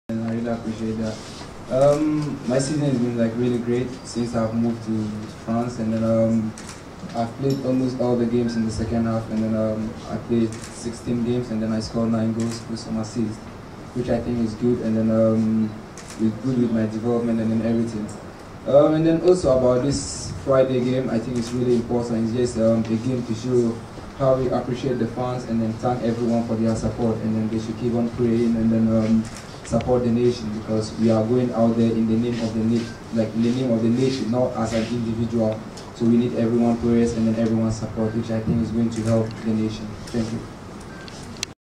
Power 103.5 FM :: Majeed Waris speaks at World Cup Farewell match presser
Ghana striker Abdul Majeed Waris calls for support from fans ahead of a farewell game in Accra for Black Stars before their departure for Brazil World Cup. The event is organized by Primeval Consult